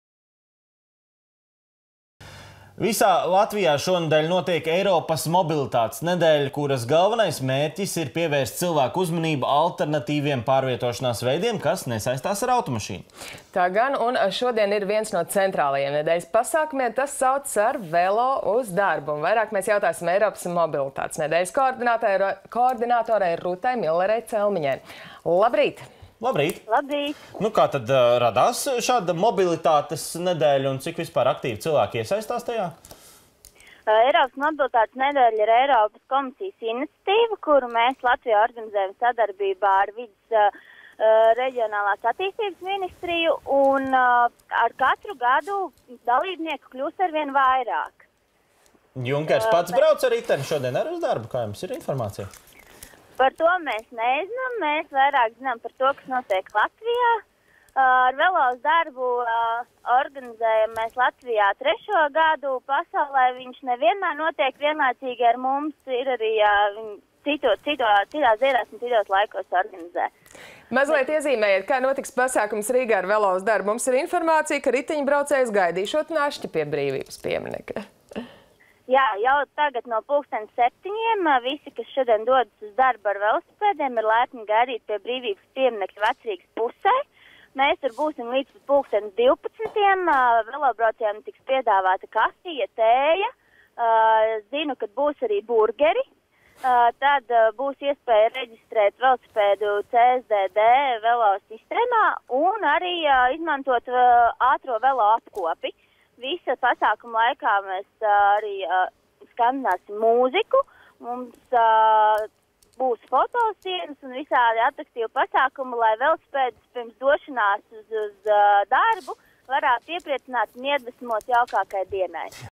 Telefonintervija ar Rutu Milleri-Celmiņu
Visā Latvijā šonedēļ notiek Eiropas Mobilitātes nedēļa, kuras galvenais mērķis ir pievērst cilvēku uzmanību alternatīviem pārvietošanās veidiem, kas nesaistās ar automašīnu.
 Šodien ir viens no centrālajiem nedēļas pasākumiem un tas saucas “Ar velo uz darbu”. Vairāk jautāsim Eiropas Mobilitātes nedēļas koordinatorei Rutai Millerei-Celmiņai.